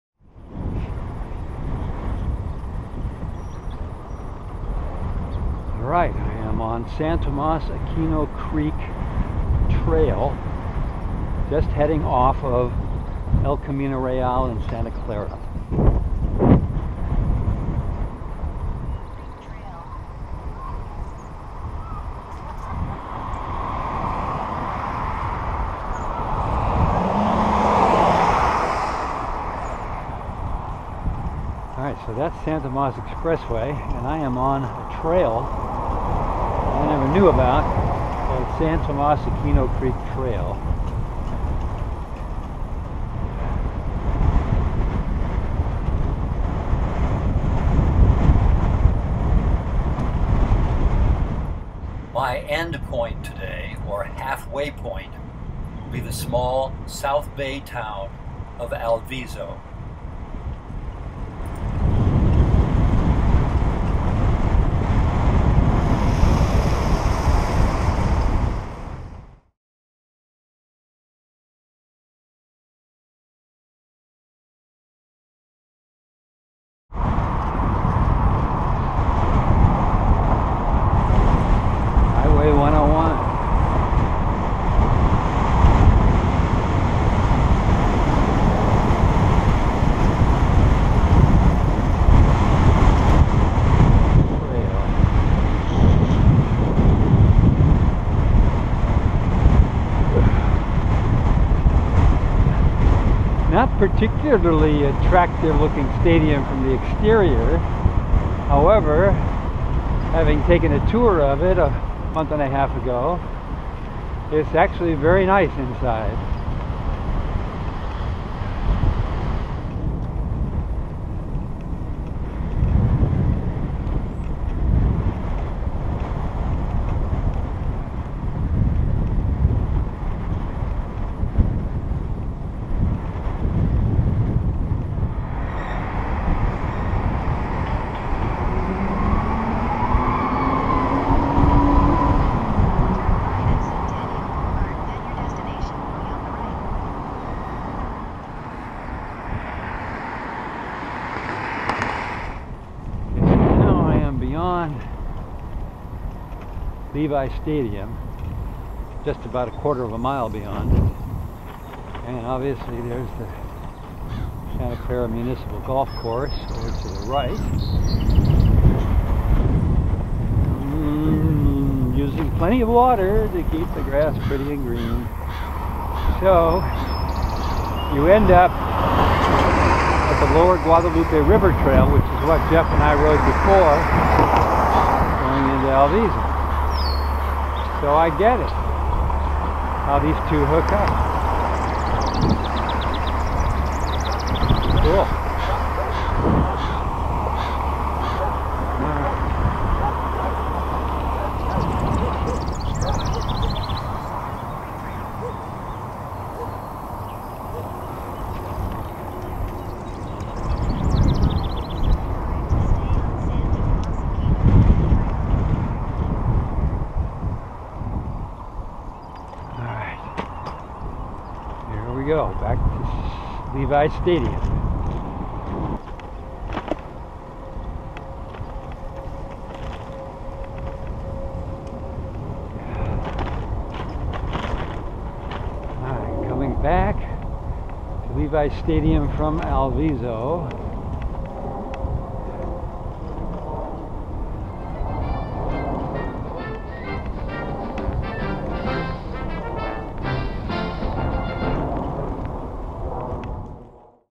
San Tomas Aquino Creek Trail, past 49ers' Levis Stadium to Alviso 4-15
This video is about San Tomas Aquino Creek Trail, passing right in front of 49ers' Levi Stadium, to Alviso 4-15